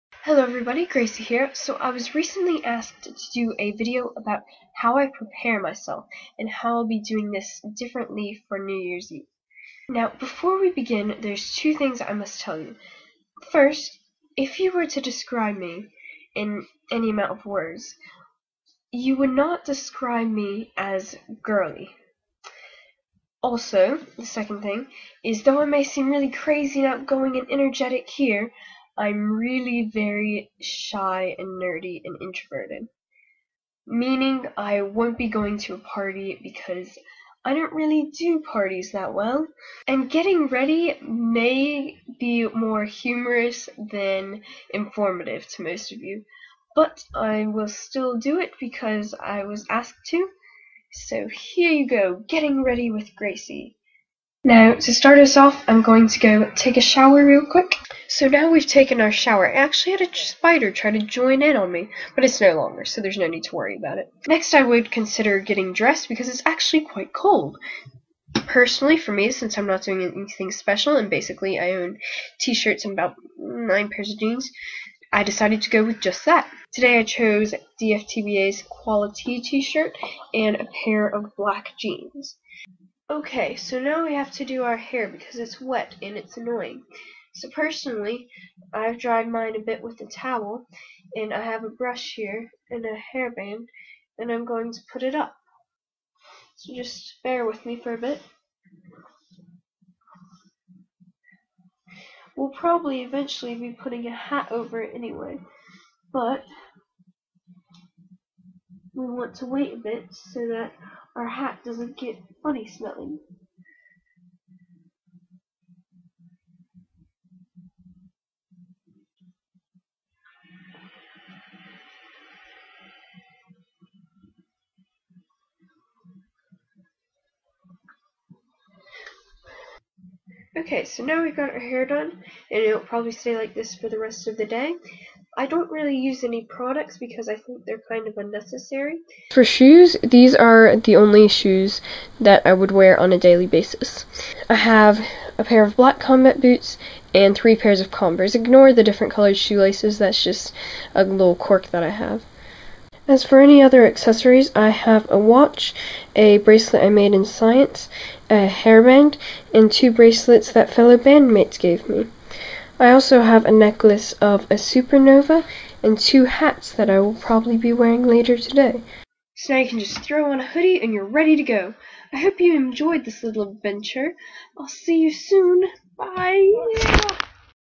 New Year's Prep
Me telling you how to get ready for New Years Eve, sort of.